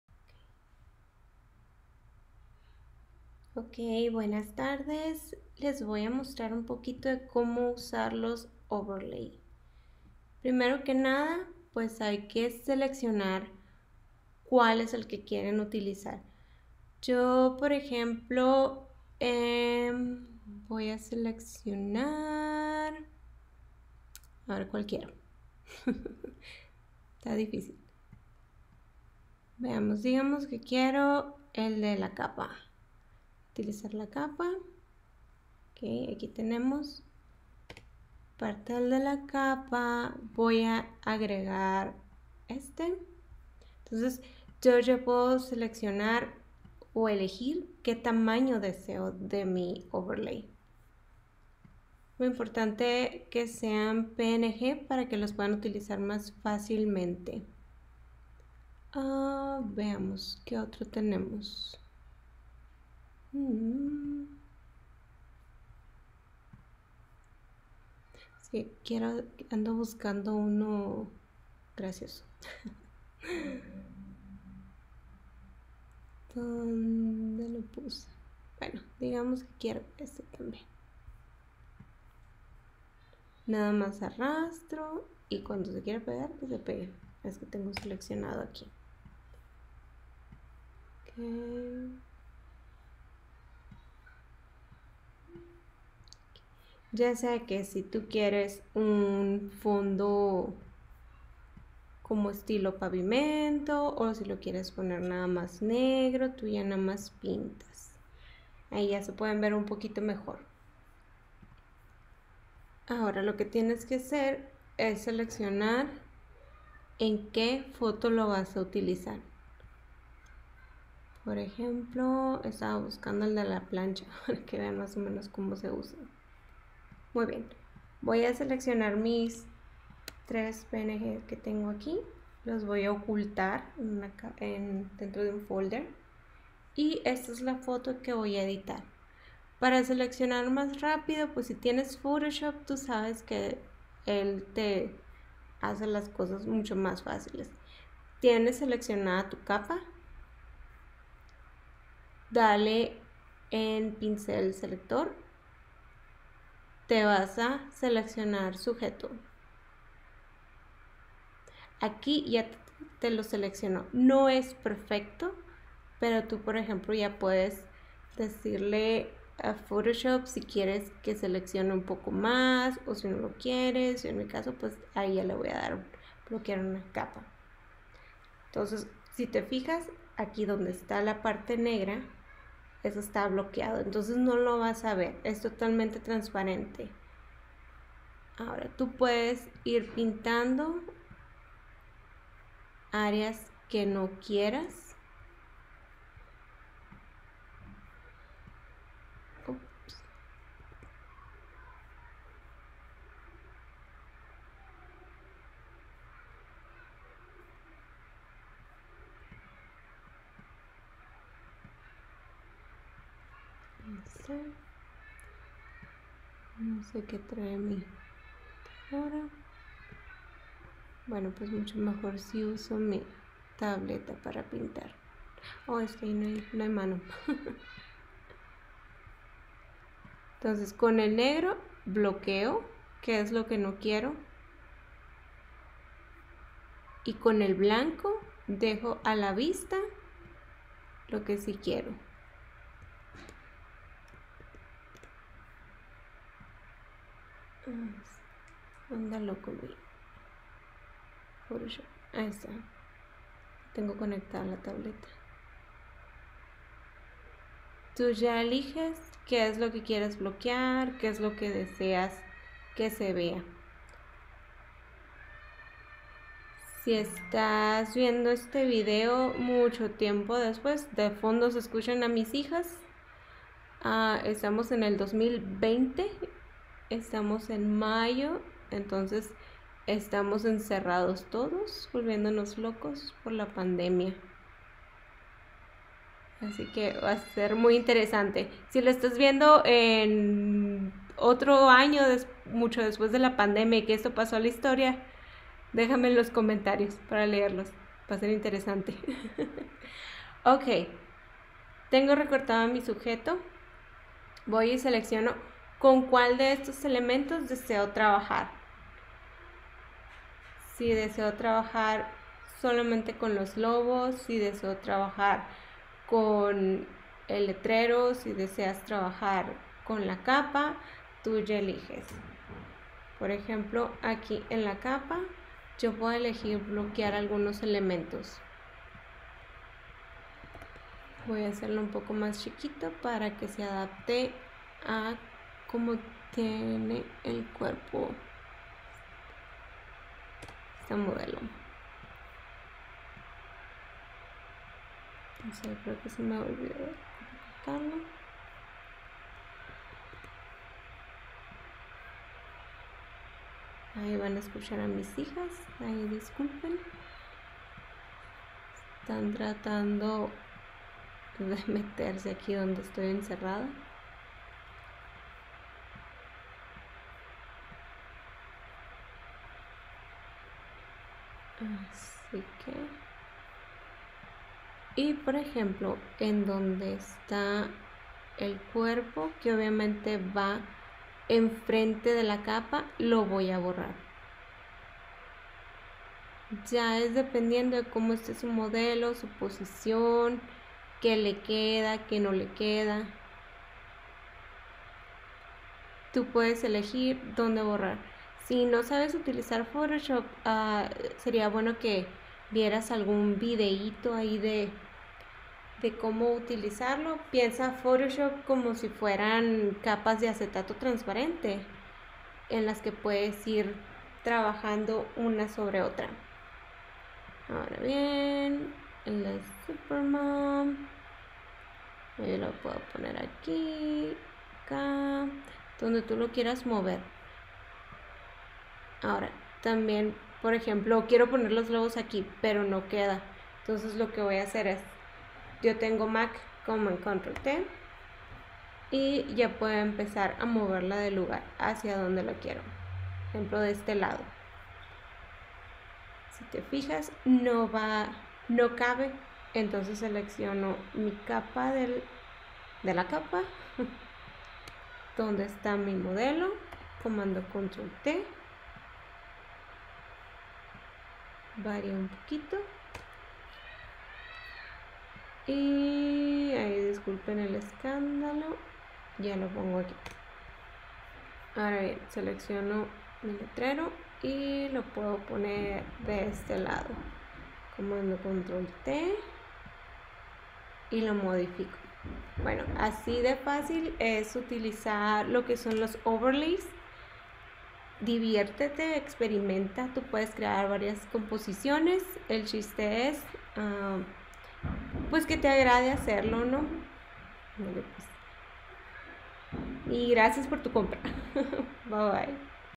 Overlay de Etsy
Un video super improvisado, en tiempos de encierro de COVID-19 en 2020 La Pandemia... jajajaja super rápido, solo para explicarles como pueden usar los overlays de ETSY